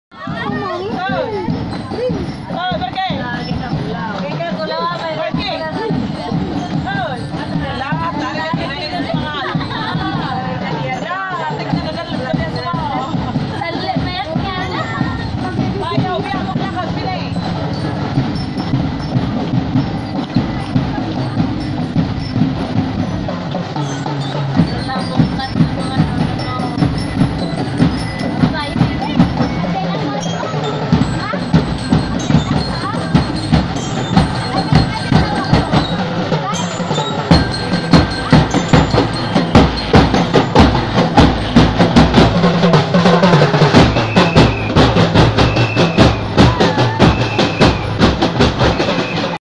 Independence Day Parade 2018
Capul, Northern Samar
06/12/2018
CAIS Drum and Bugle Corps